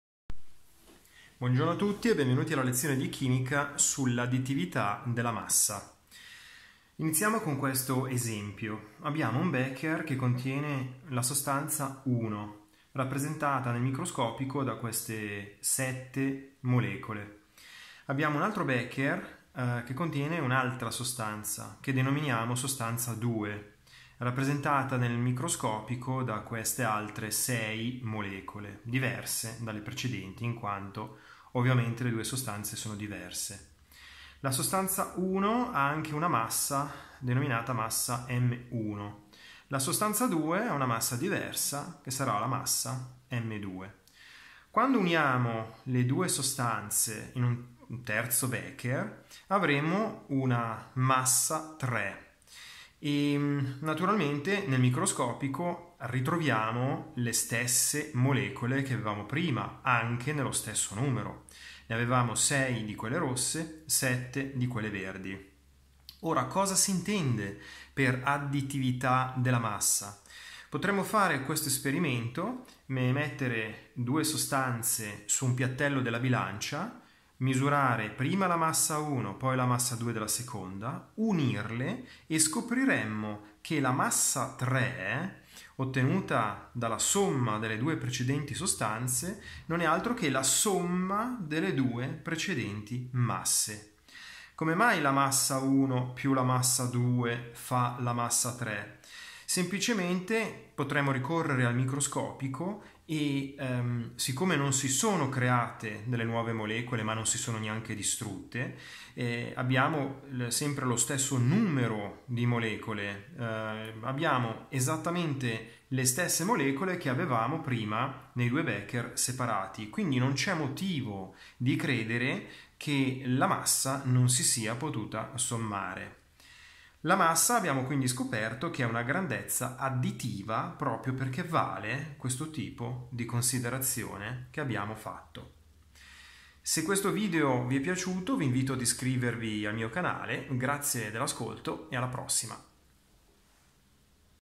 Additività della massa di sostanze diverse (la massa è una grandezza additiva) - lezione di chimica
Lezione di chimica in cui si discute dell'additività delle masse di sostanze diverse ricorrendo al modello microscopico. Detto altrimenti, si dimostra che la massa è una grandezza additiva.

massa sostanza 1 + massa sostanza 2 = massa 3 (massa del miscuglio dato dalla somma delle due precedenti masse)